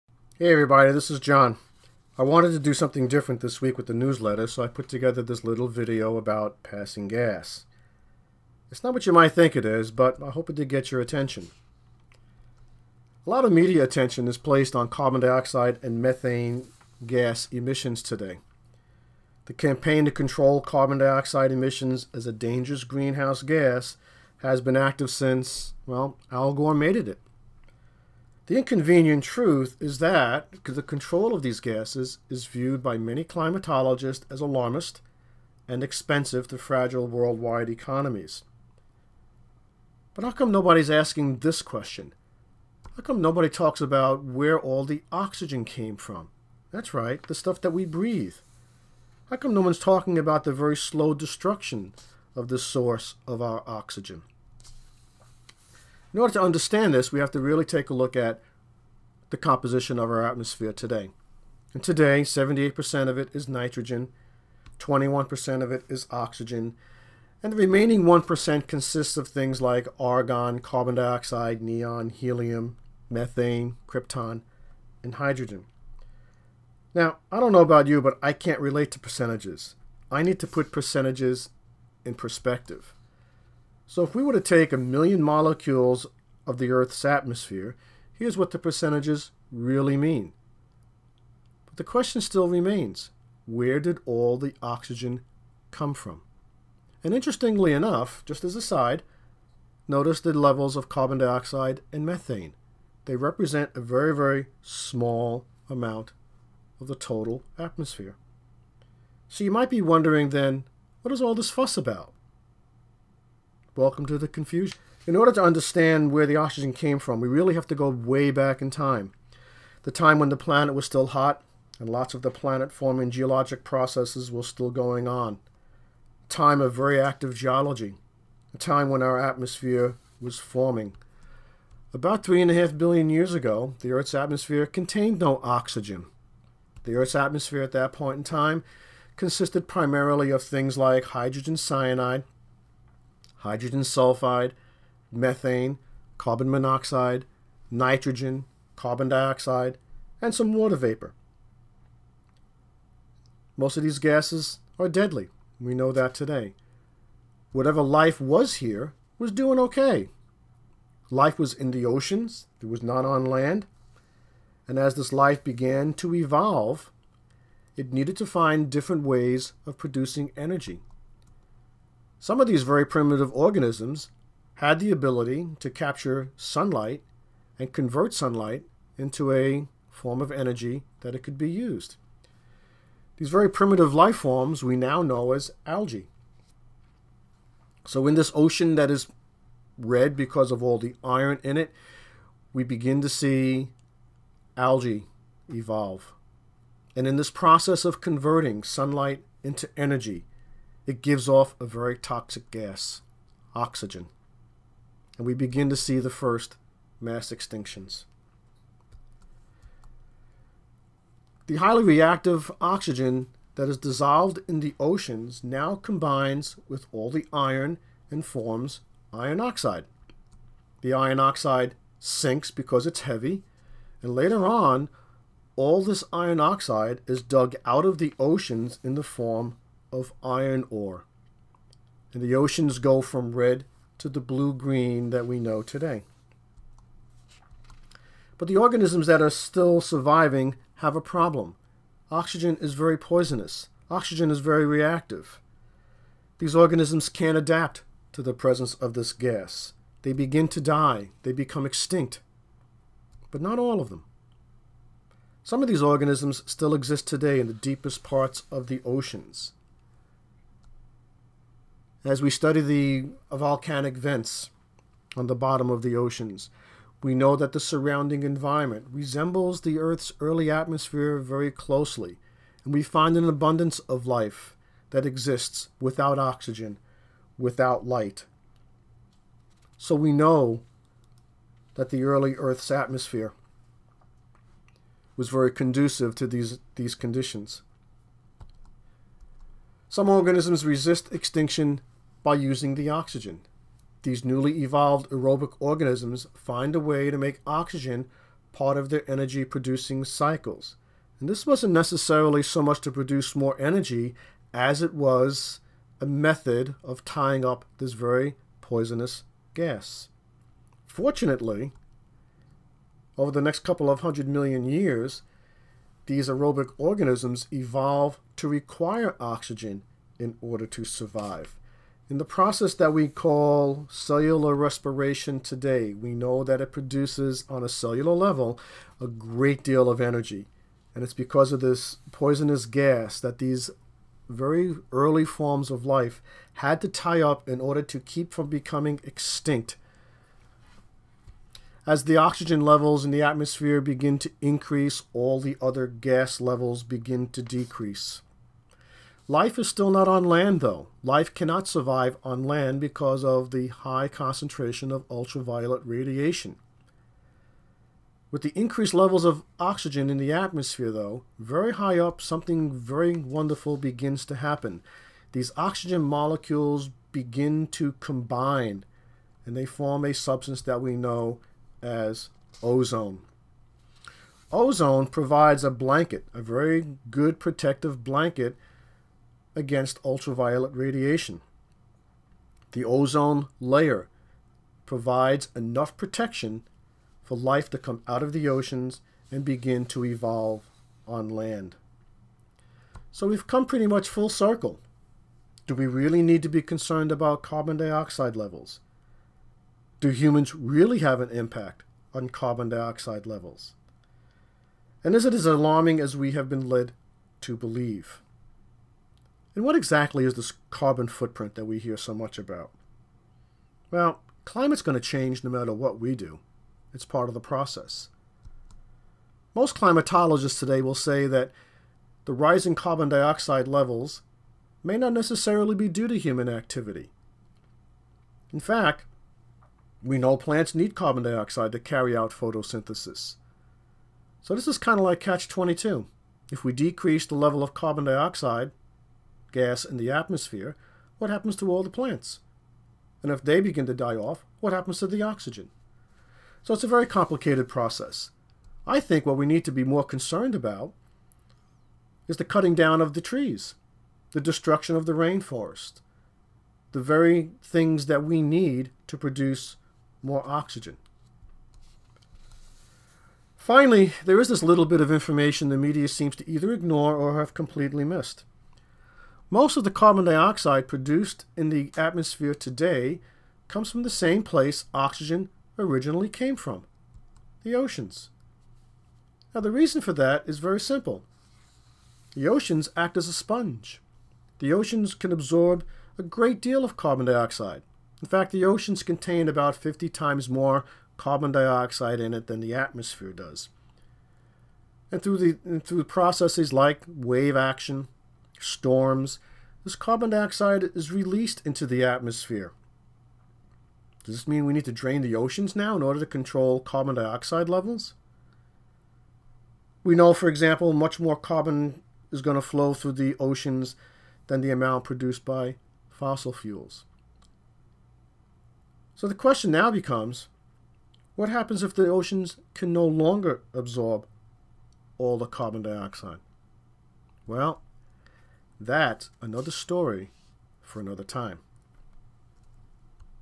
Passing Gas: Carbon Emissions, Poisonous Oxygen, and Global Warming
Debate rages about carbon emissions and global warming. Forget carbon. Where did the OXYGEN come from?